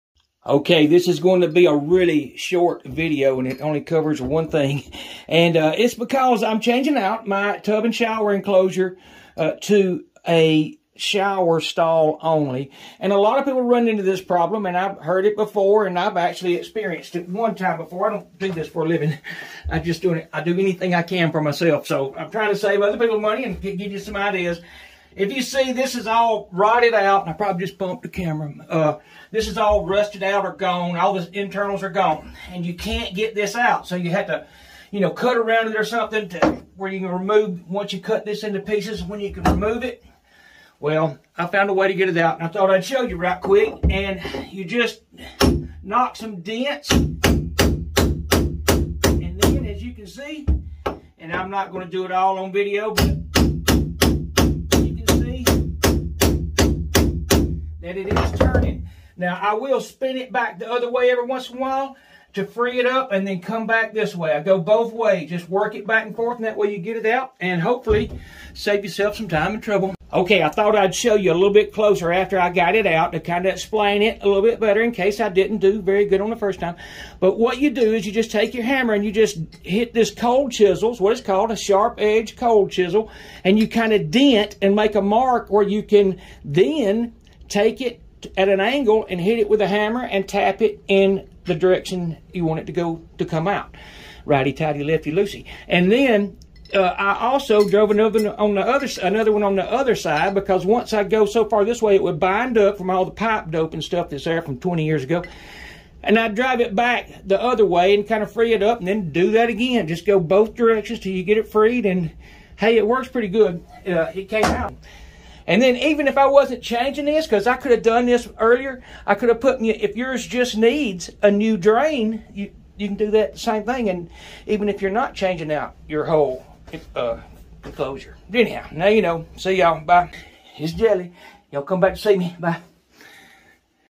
How to Replace Broken Tub or Shower Drain- simple method using cold chisel
how to remove a tub or shower drain that the middle has rusted or fallen out...